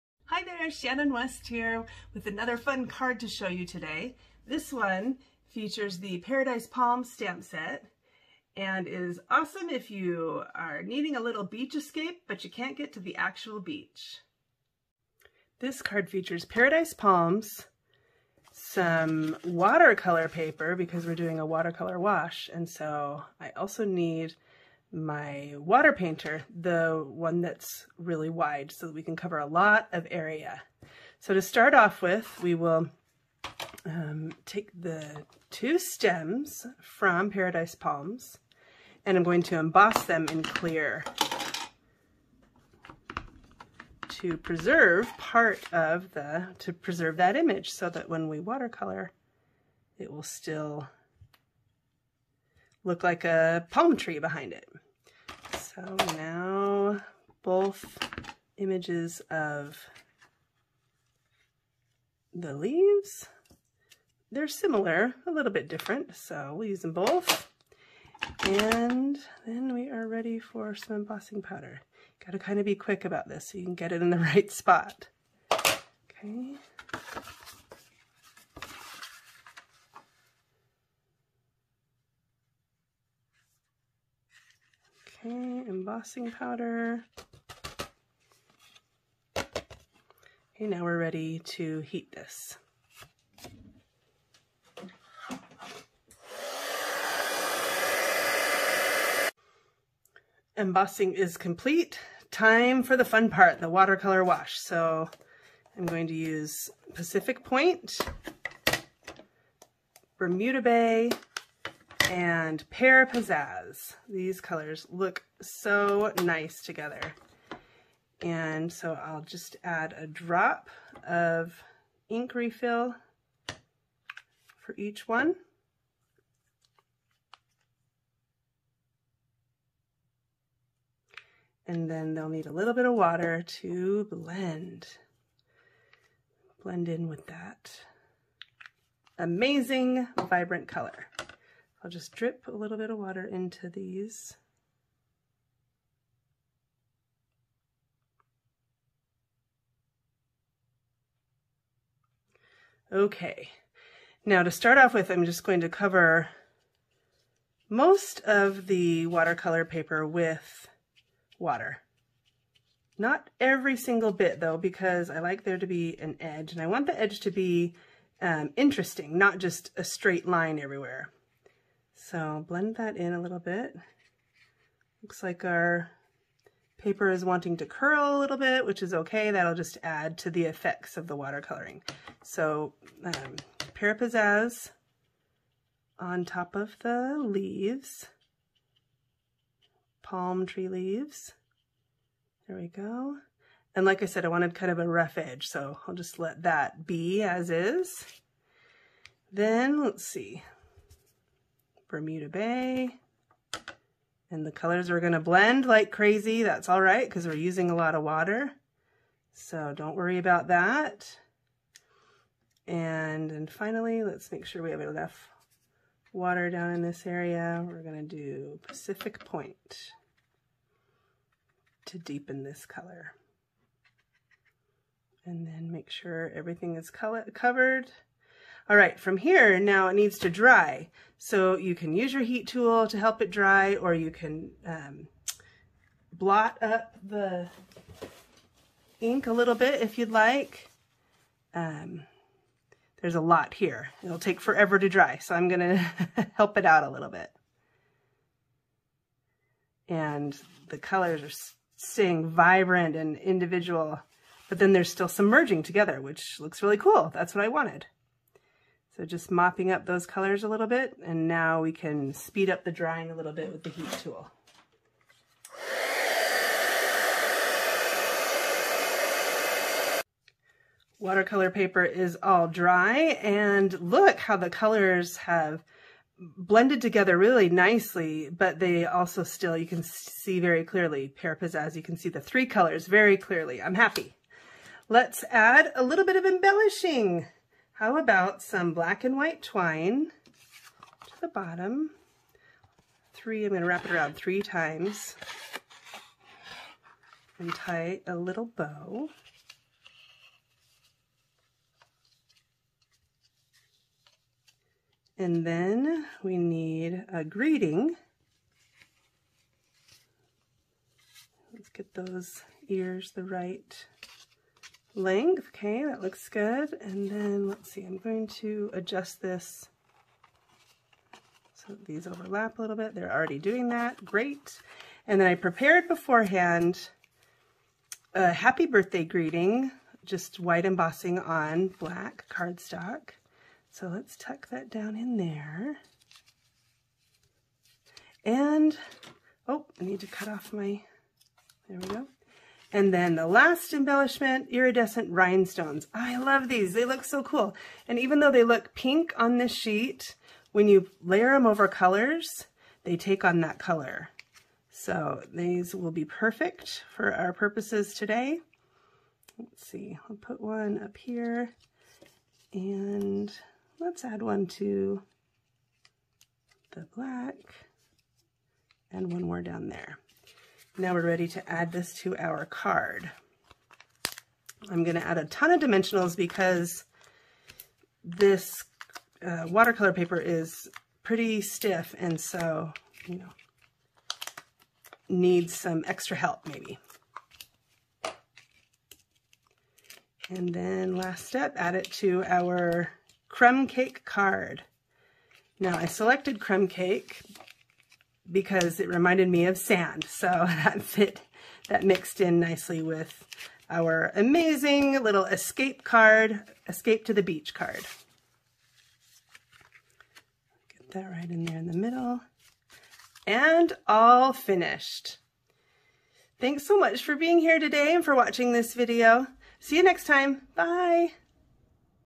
Watercolor Wash Card with Embossed Palm Trees
Watercolor washes are so fun! Start with embossing an image beforehand, then add a watercolor wash over the top. Not sure how to do this? Watch and learn!